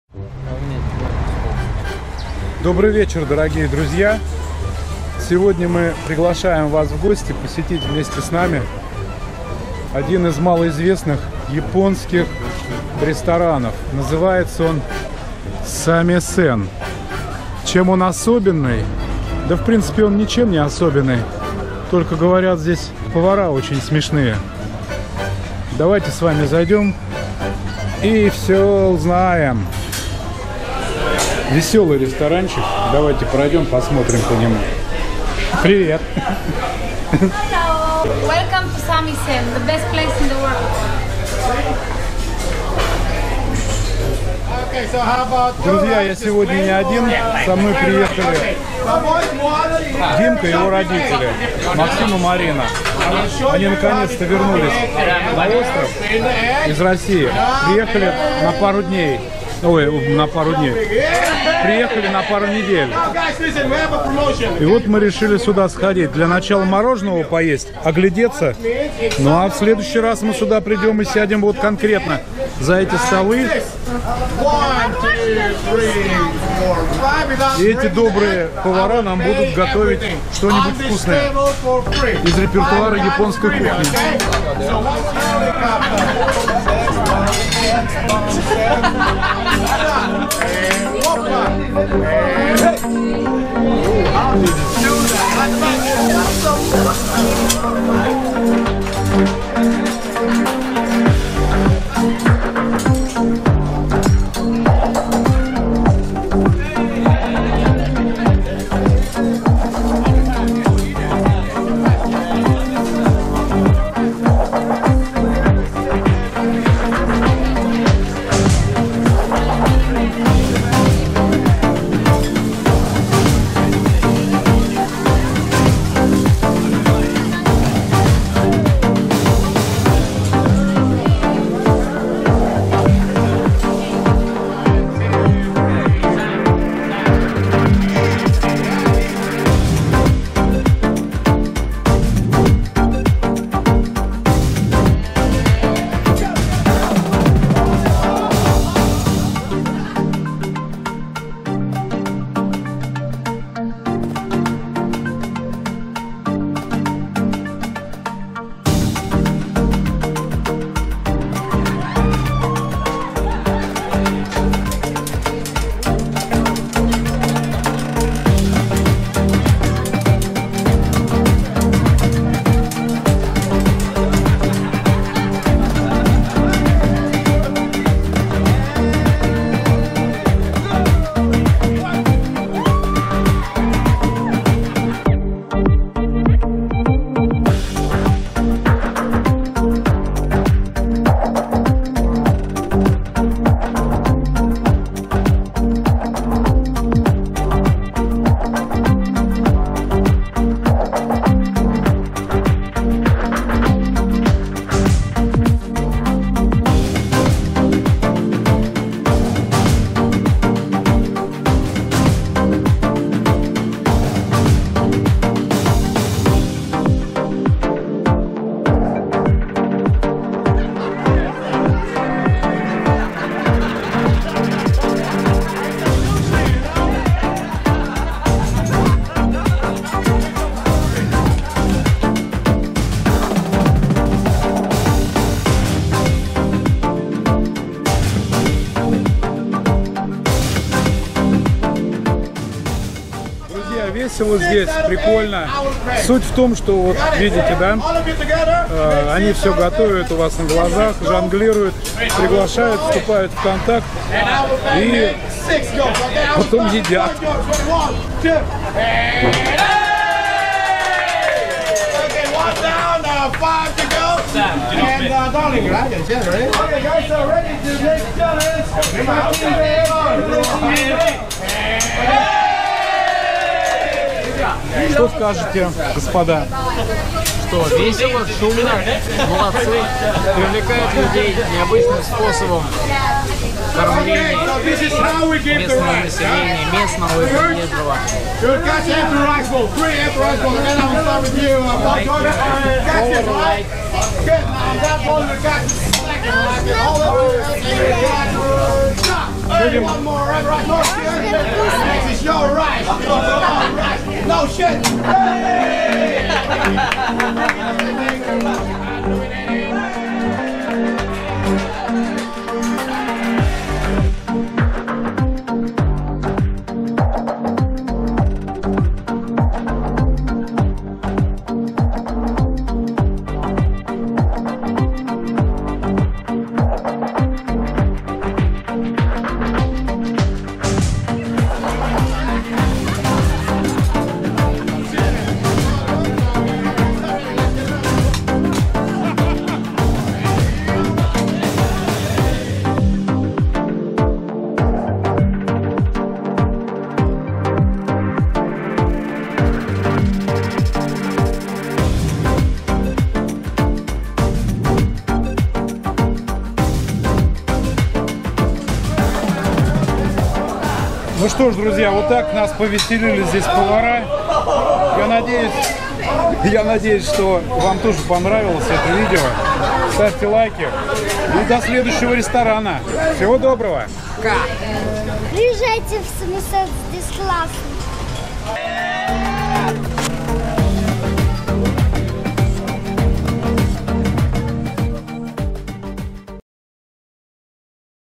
Японская кухня. Ресторан SAMISEN в Корал Бей. CYPRUS. Samisen Restaurant at Coral Bay
♚ ССЫЛКИ НА СОЦ. СЕТИ ♚
✔ Твиттер ►

РУССКИЕ В КИПРЕ. Ресторан SAMISEN в Корал Бей расположен в 10 километрах от центра Пафоса. Очень весёлые повара ресторана не дадут вам скучать. Вы примите непосредственное участие в приготовлении блюд.